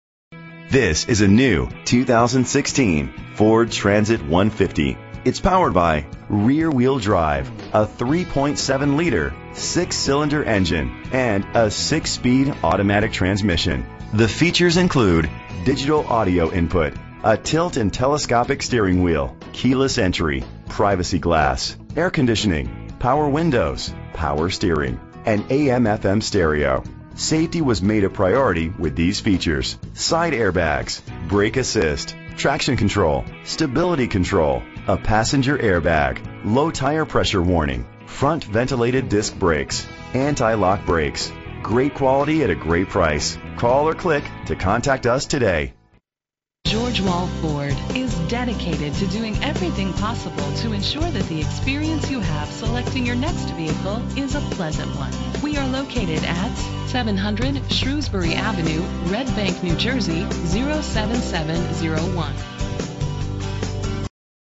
2016 Ford Transit-150 - Red Bank NJ
Phone:
Year: 2016
Make: Ford
Model: Transit-150
Trim: 130 WB Low Roof Cargo
Engine: 3.7 liter 6 cylinder 24 valve
Transmission: 6-Speed Automatic
Color: Oxford White
Mileage: 0
Address:
700 Shrewsbury Ave.